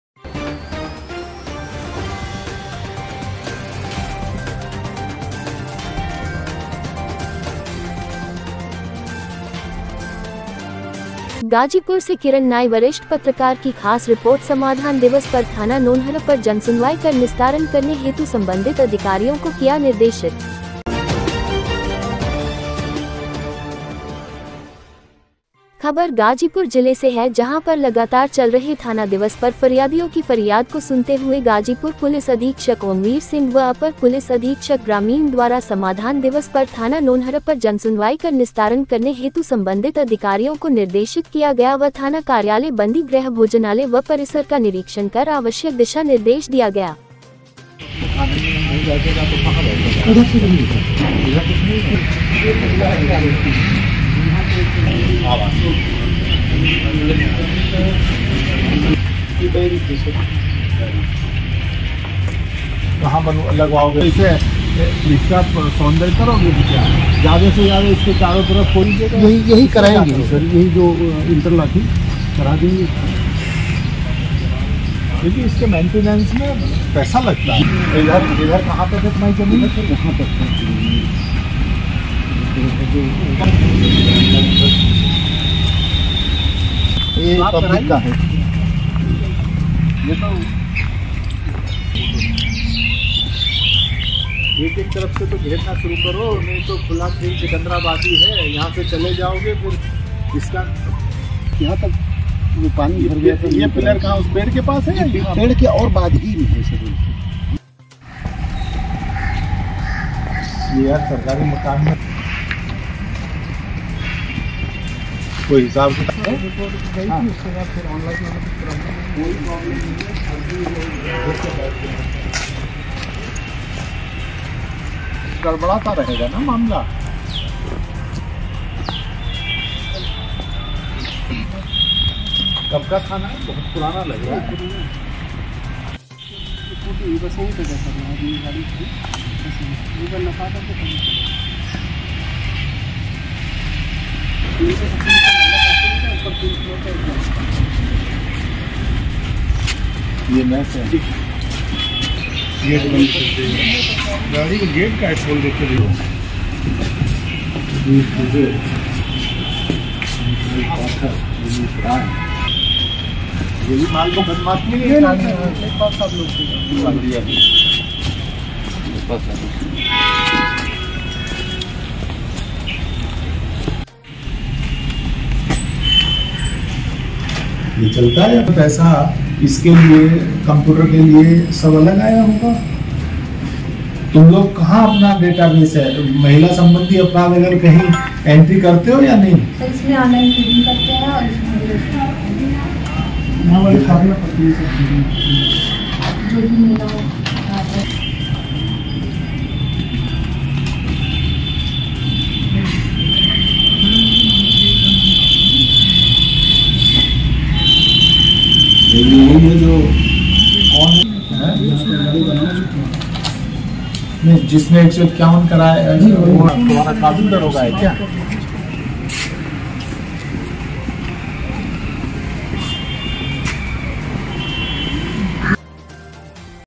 गाजीपुर -समाधान दिवस पर थाना नोनहरा पर!...जनसुनवाई कर निस्तारण करने हेतु संबंधित अधिकारियों को किया
गाजीपुर -समाधान दिवस पर थाना नोनहरा पर!...जनसुनवाई कर निस्तारण करने हेतु संबंधित अधिकारियों को किया निर्देशित।